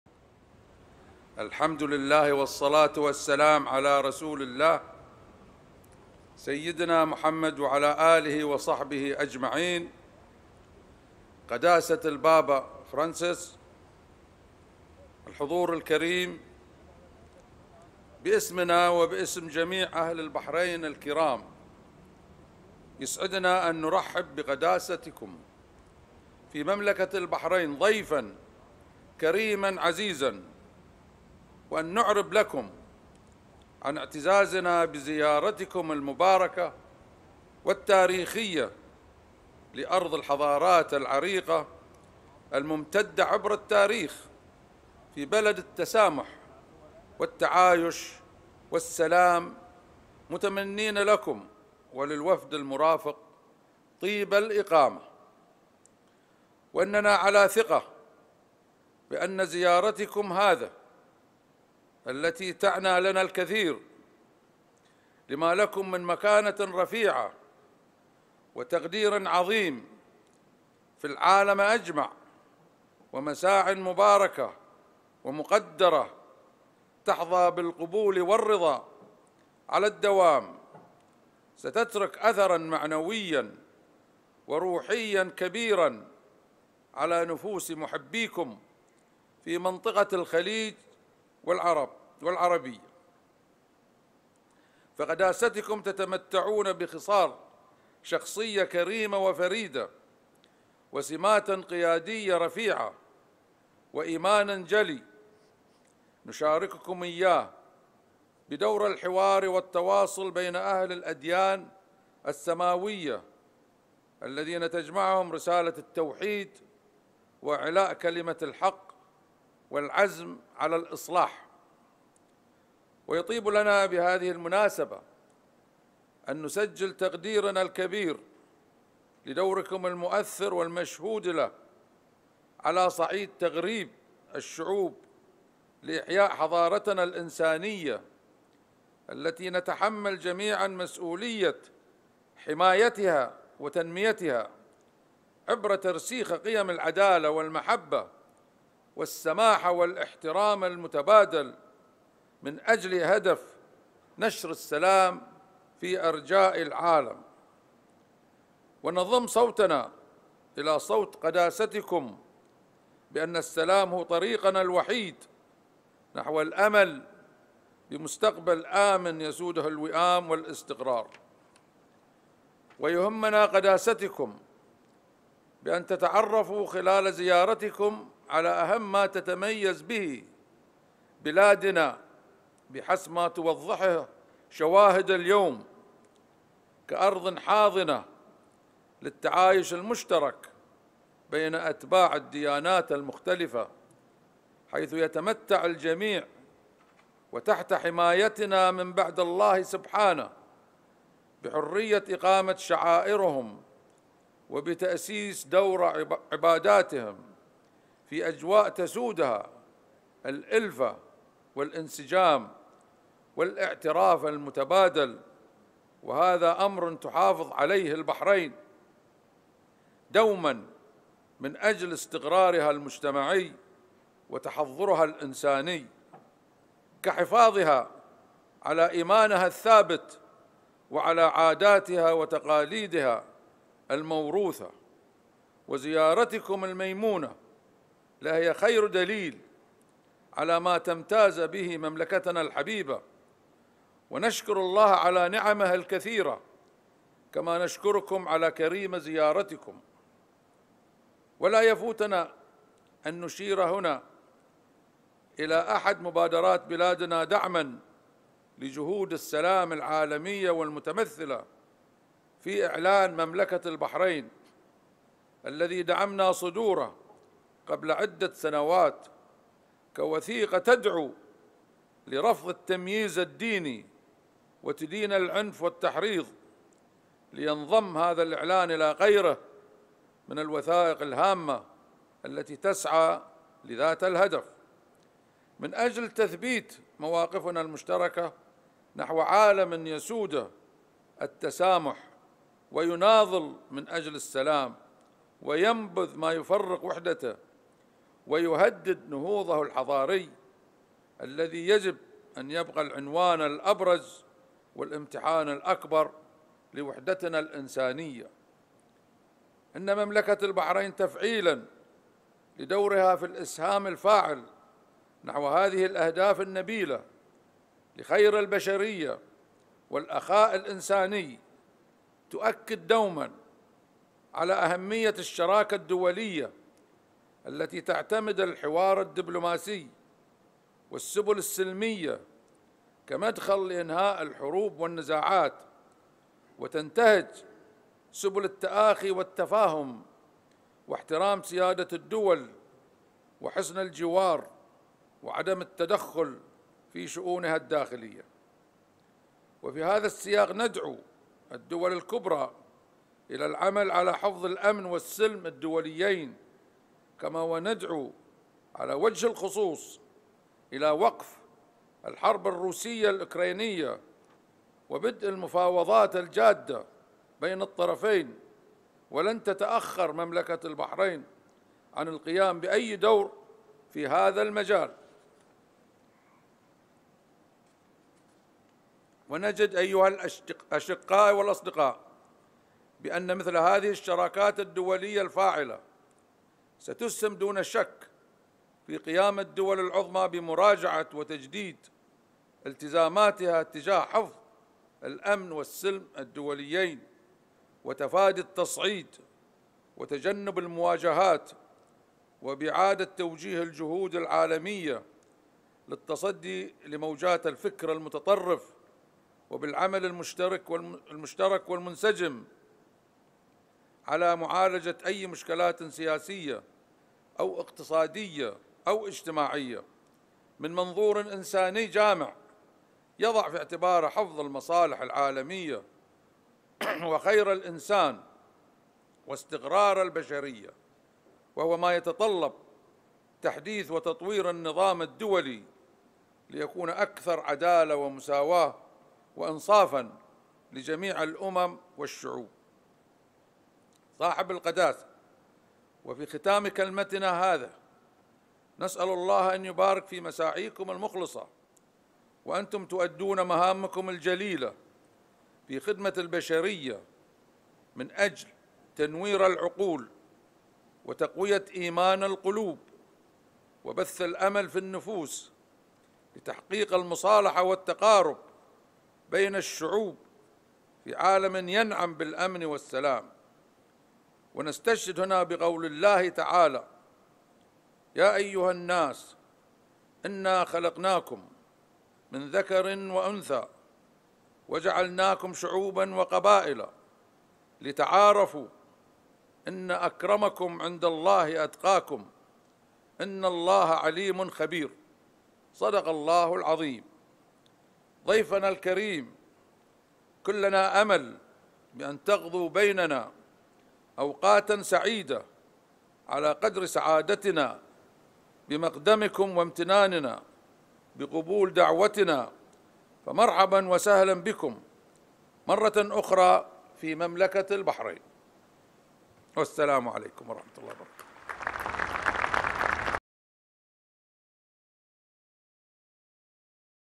العاهل البحريني: واثقون من أن زيارة بابا الفاتيكان ستترك أثرا معنويا
غادر البابا_فرانسيس روما اليوم متوجها إلى البحرين في إطار رحلة شرق أوسطية.
وتعد الزيارة الثانية التي يقوم بها البابا فرنسيس إلى منطقة الخليج بعد رحلته إلى الإمارات في 2019.
ومن المقرر أن يلقي البابا خلال تواجده في البحرين كلمة أمام "منتدى حوار البحرين: الشرق والغرب للتعايش البشري"، الذي ينظمه مجلس حكماء المسلمين في دولة الإمارات، يليه لقاءٌ خاص مع شيخ الأزهر أحمد الطيب.